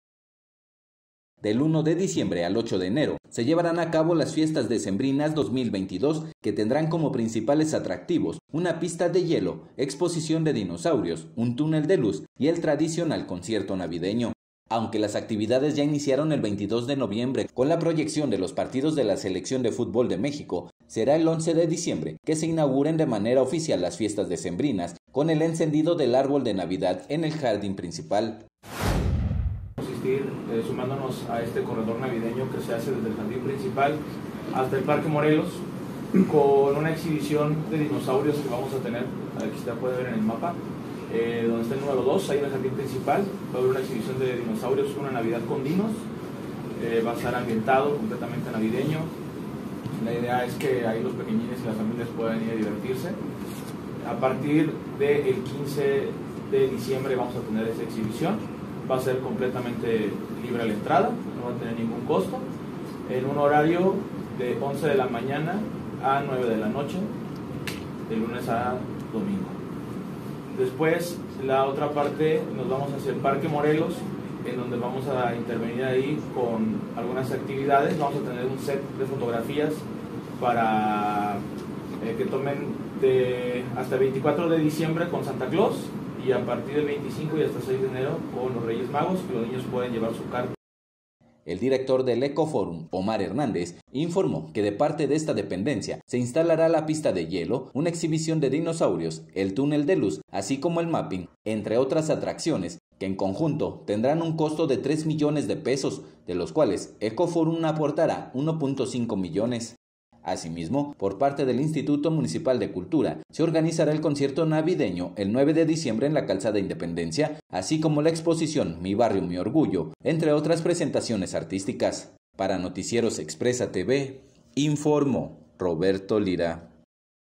🔴📽️🌲⛄ Presentan cartel de Fiestas Decembrinas
Del uno de diciembre al ocho de enero se llevarán a cabo las Fiestas Decembrinas 2022, que tendrán como principales atractivos una pista de hielo, exposición de dinosaurios, túnel de luz y el tradicional concierto navideño.
Aunque las actividades ya iniciaron el 22 de noviembre con la proyección de los partidos de la Selección de futbol de México, será el 11 de diciembre que se inauguren las fiestas decembrinas con el encendido del árbol de navidad en el jardín principal.